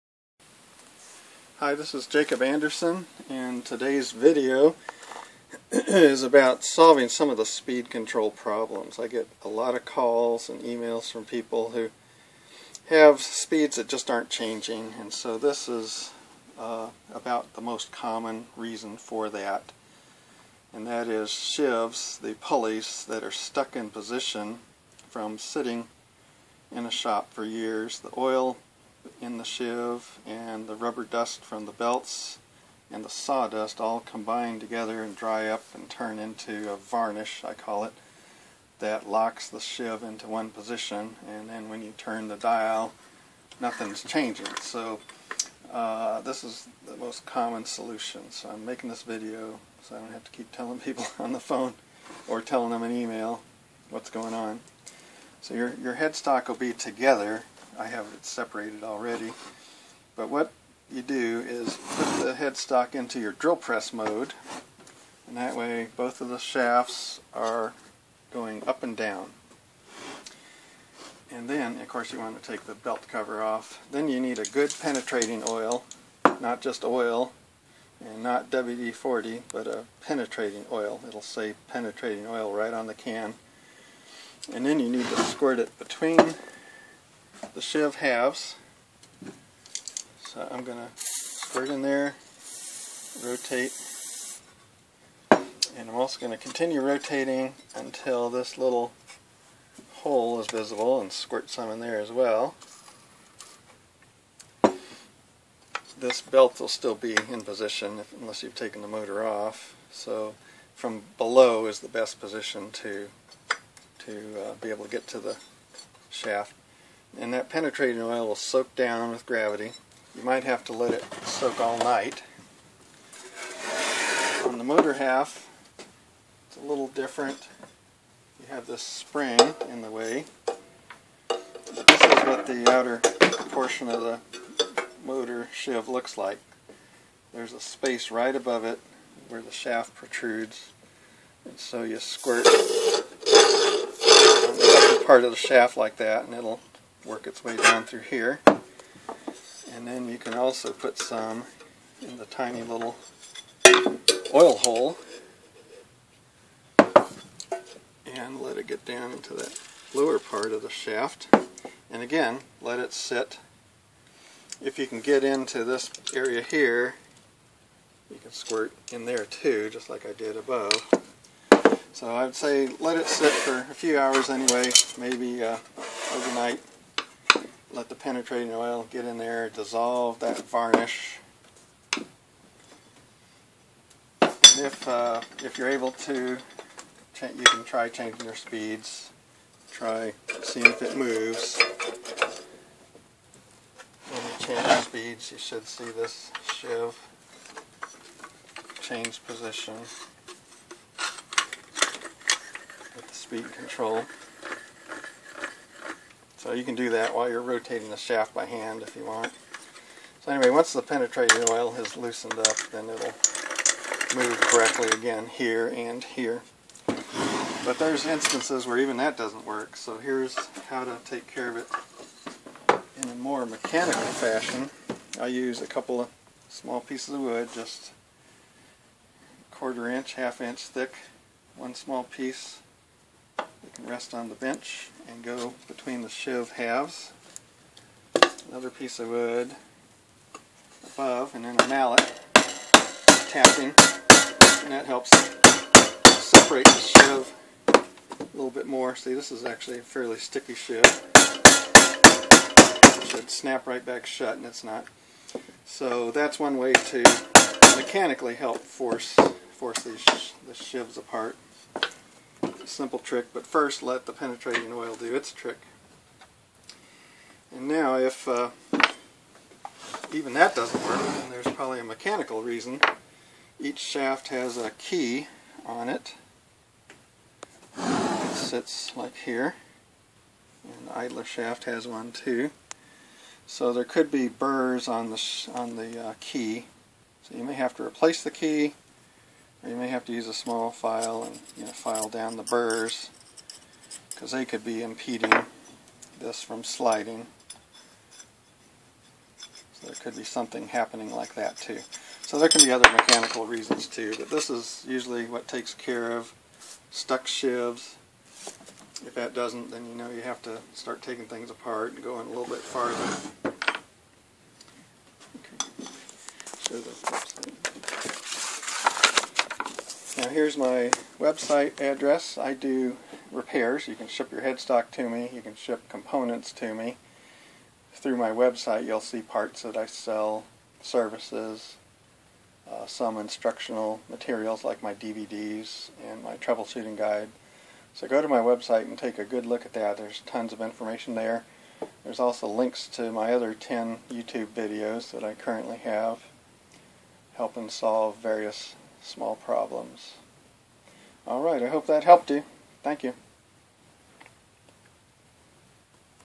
Shopsmith Mark V speed control problems - sheaves
Jacob Anderson, the nation's leading independent Shopsmith Mark V headstock repair technician, shows how to solve the most common speed control problem, stuck sheaves which will not allow speeds to change.